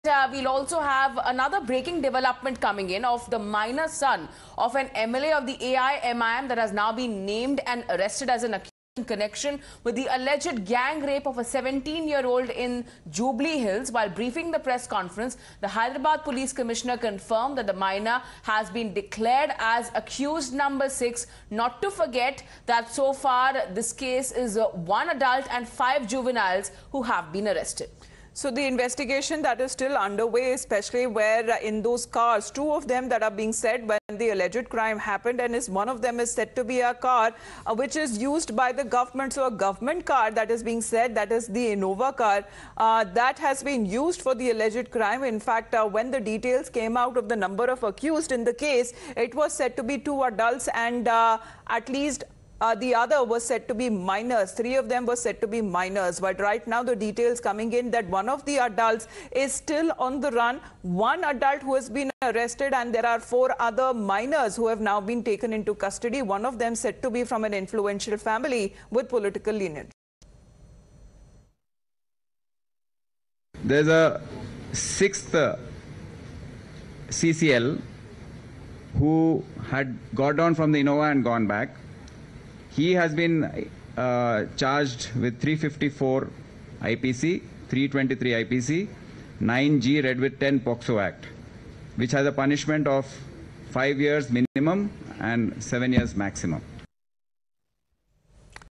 Hyderabad Horror | AIMIM's MLA Minor Son Confirmed As 6th Accused, Govt Car Used For Alleged Crime
The minor son of an All India Majlis-e-Ittehadul Muslimeen (AIMIM) MLA has been confirmed as the 6th accused in the Hyderabad minor gangrape that took place in Hyderabad's Jubilee Hills area on 28 May. All six accused- five of them juveniles- are now in police custody. Earlier, the police said that the MLA's son was not involved in the gangrape. This sparked outrage from the opposition parties which accused the police of trying to shield the AIMIM's MLA son. A government Innova car is said to be used in the alleged crime.